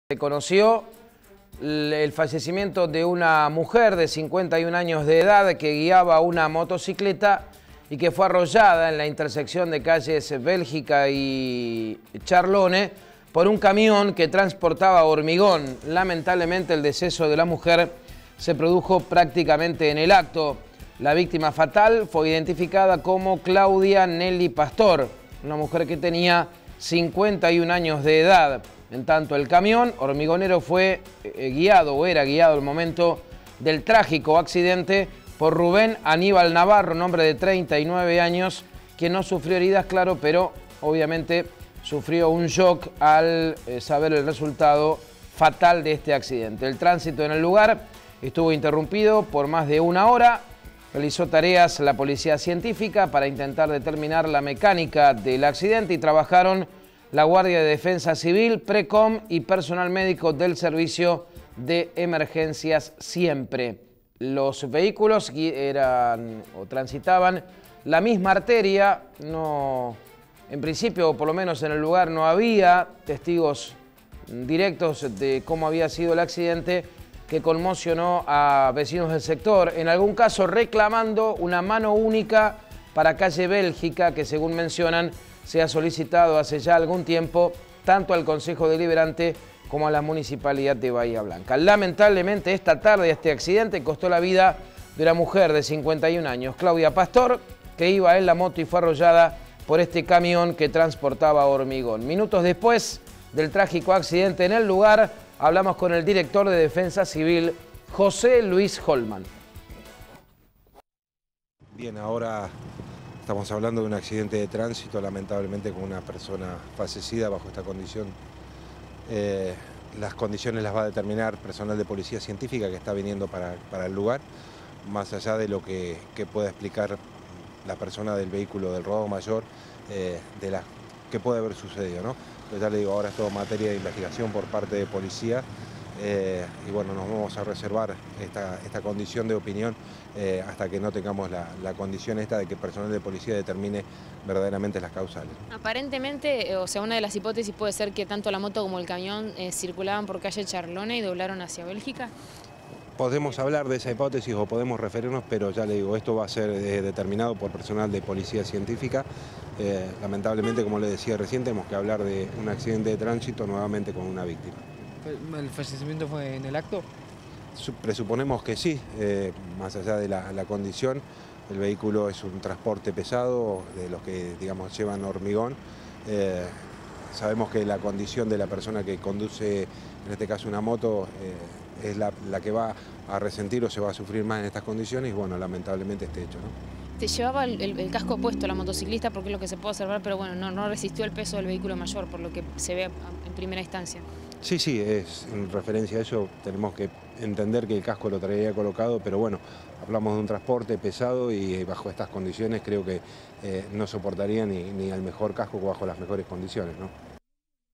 Accidente fatal en Bélgica y Charlone
Una mujer iba en moto y fue arrollada por un camión que cargaba hormigón. Murió en el acto.